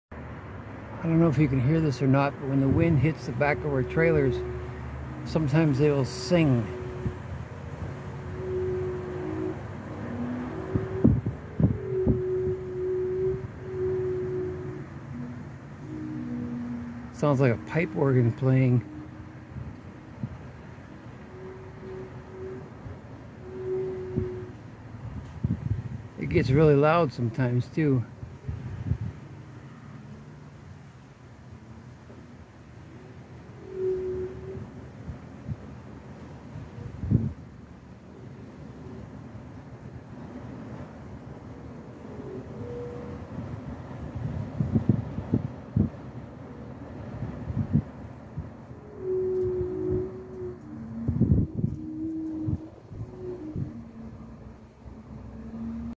TURN THE VOLUME UP
WIND MAKES PIPE ORGAN SOUND ON THE BACK OF MY TRAILER